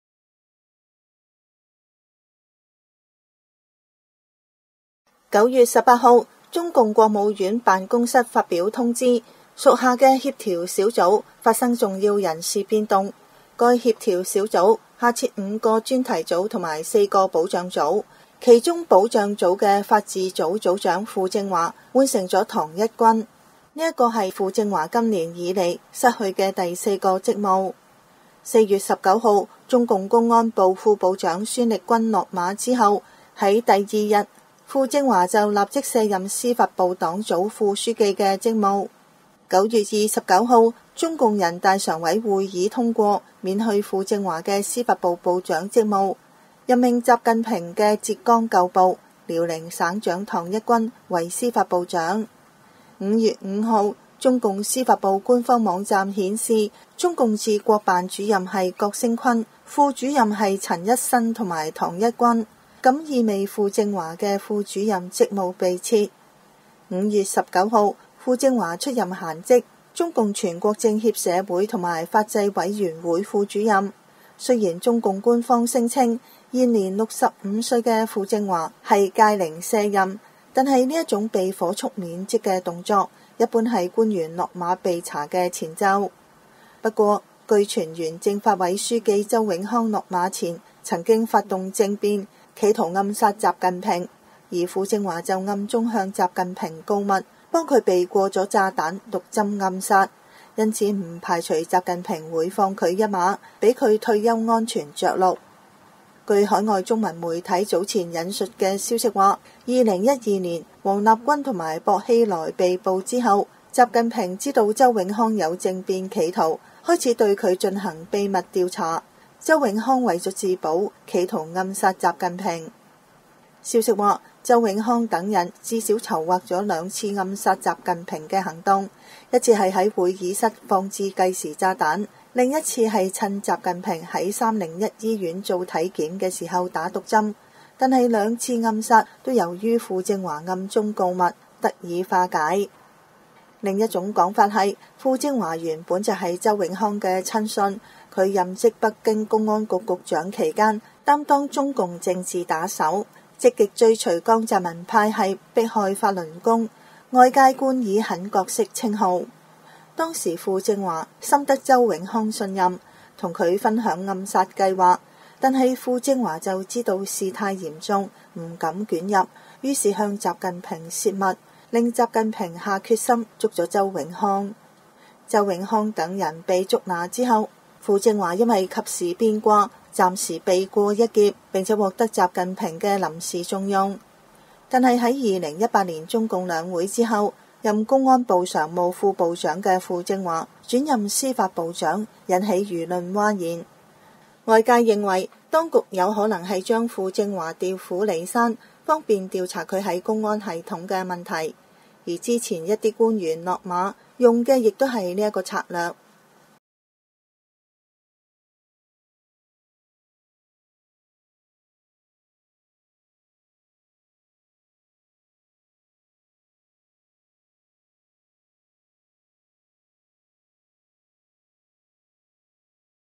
【焦點速遞】今年，中共司法部前部長傅政華接連失去4個職務，被認為是低調落馬表現。傳傅政華曾兩次幫習躲過江派暗殺行動，不排除習放其一馬，但傅結局如何尚難預料
9月18日，中共國務院辦公室發佈通知，下屬的「協調小組」發生重要人事變動。該協調小組下設5個專題組和4個保障組。其中，保障組中法治組的組長傅政華，換成了唐一軍。這是傅政華今年以來失去的第四個職務。

4月19日，中共公安部副部長孫力軍落馬後，次日，傅政華即卸任司法部黨組副書記的職務。

4月29日，中共人大常委會議通過免去傅政華的司法部部長職務，任命習近平的浙江舊部、遼寧省長唐一軍為司法部部長。

5月5日，中共司法部官網顯示，中共治國辦主任為郭聲琨，副主任為陳一新、唐一軍。這意味著傅政華的副主任職務被撤。

5月19日，傅政華出任閒職——中共全國政協社會和法制委員會副主任。

雖然中共官方聲稱，現年65歲的傅政華是屆齡卸任。但這種被火速免職的動作，一般是官員落馬被查的前奏。

不過，據傳原政法委書記周永康落馬前曾發動政變，企圖暗殺習近平，傅政華暗中向習近平告密，幫習躲過了炸彈、毒針暗殺。因此不排除習會放他一馬，讓他退休安全著陸。

據海外中文媒體此前援引消息稱，2012年王立軍、薄熙來被捕後，習近平知曉周永康有政變企圖，開始對周進行秘密調查。周永康為自保，企圖暗殺習近平。

消息稱，周永康等人至少籌劃了兩次暗殺習近平的行動：一次是在會議室置放定時炸彈，另外一次是趁習近平在301醫院做體檢時打毒針。但兩次暗殺皆由於傅政華暗中告密，得以化解。

另有一種說法是，傅政華原本就是周永康的親信，他任職北京公安局局長期間擔當中共「政治打手」，積極追隨江派迫害法輪功，被外界冠以「狠角色」之稱。

當時傅政華深得周永康信任，同他「分享」了暗殺計劃。但傅知道事態嚴重，不敢捲入，於是向習近平「洩密」，使習近平下決心抓了周永康。

周永康等人被拿下後，傅政華因及時「反水」暫時躲過一劫，並獲得習的臨時重用。但在2018年中共兩會後，任公安部常務副部長的傅政華轉任司法部部長，引起輿論嘩然。

外界認為，當局有可能是將傅政華調虎離山，以方便查他在公安系統的問題。此前一些官員落馬，用的也是這個策略。

責任編輯：annie hsu

——滅共大潮下，美軍施壓，中美將打仗⚔️？中共推數字貨幣💰洗劫財富
習近平與曾慶紅生死大戰 ㊙！